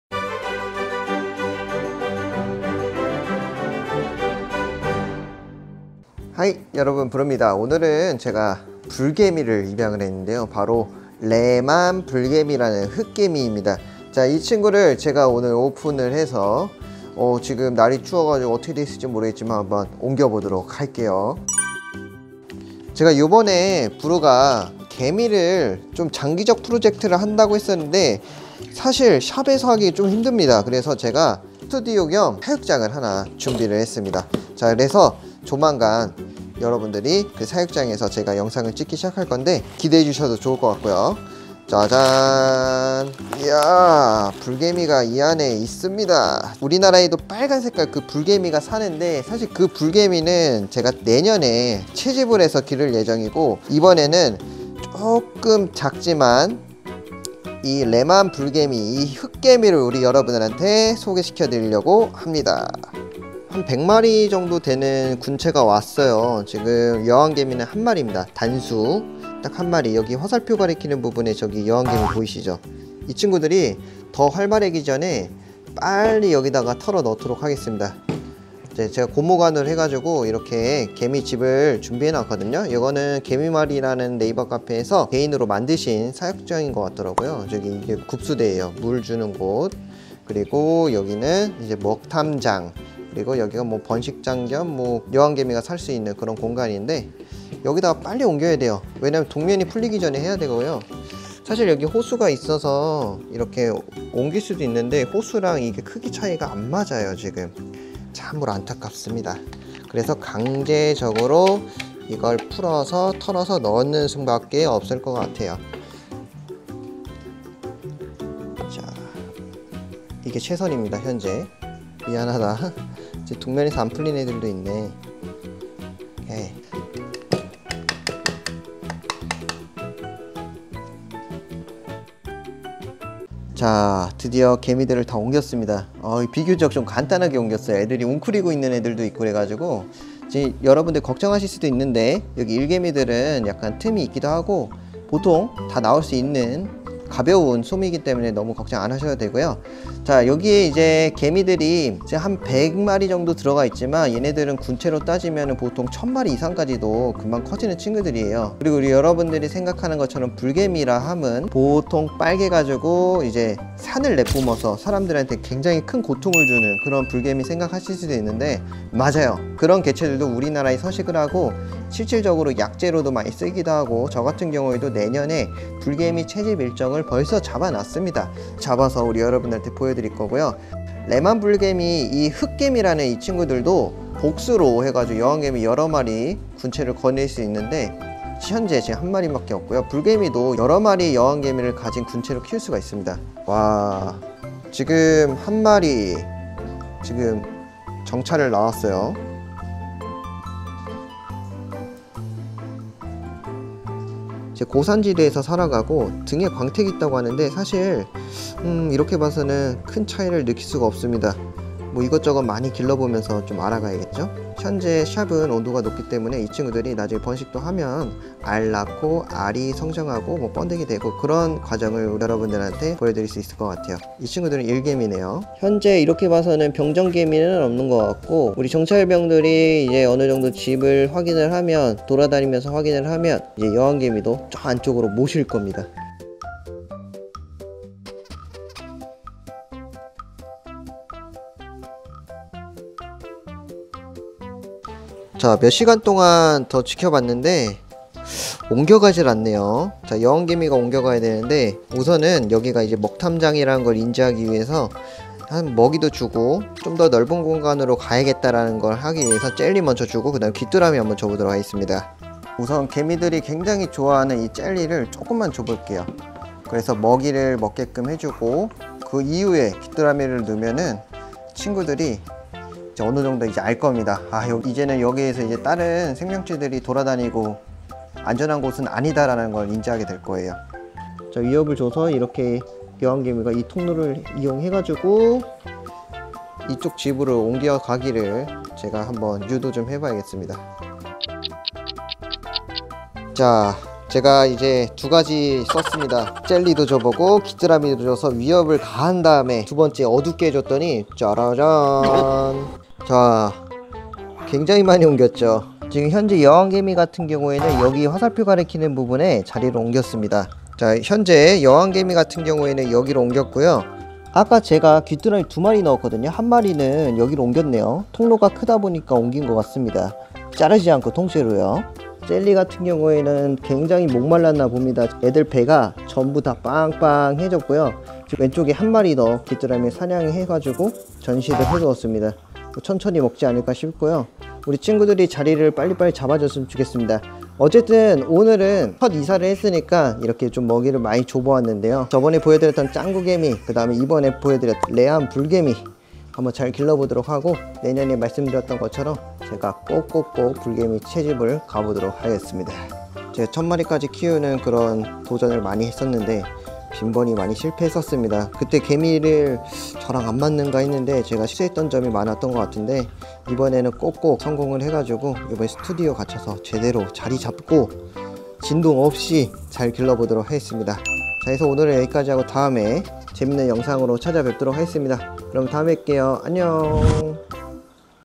여왕개미폐사 사육종료)불개미군체가 도착했다!
레만불개미(흑개미)는 이름이 불개미라는 이름이 왜 붙여졌는지 잘모르겠지만
한두마리 탈출했을때 저를굉장히 강하게 공격하려는 성향을 봐서는 말그대로 BULL(황소) 처럼 비슷한 거친 성격을 가진듯 합니다

불개미는 불처럼 빨개서 불개미가 아닙니다. 성격 때문에 그래요!
레만불개미는 광택이 좋고 고산지대에 서식하는 종으로써 한국에 1900년대에 발견된 개미입니다!

브르가 잘 키워볼게요. 여러분들이 생각하는 불개미! 채집하러 가볼게요 ㅋㅋ